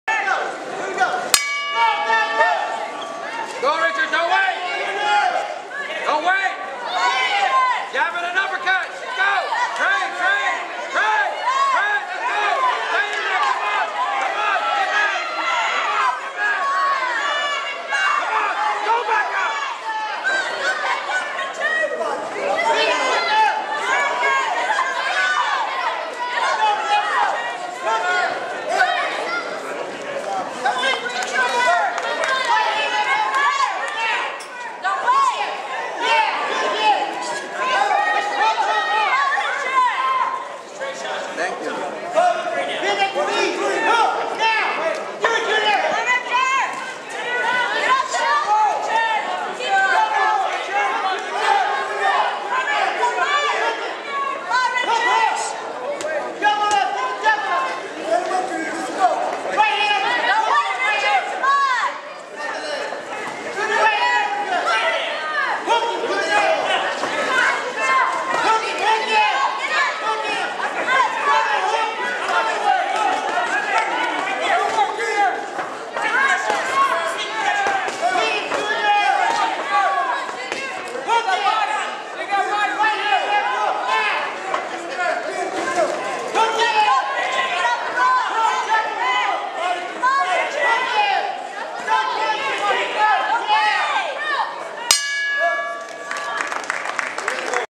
Richard Acevedo vs JR Rodriguez Rd 2 (2 of 4)
Kansas-Oklahoma Regional Golden Gloves finals at the Memorial Hall  in Hutchinson, Kansas. March 27, 2010. Junior division 122 lbs contest. Richard Acevedo (red headgear) of Bad Boyz Boxing club in Garden City, Kansas. Adan 'JR' Rodriguez of Northside 316 Boxing in Wichita, Kansas.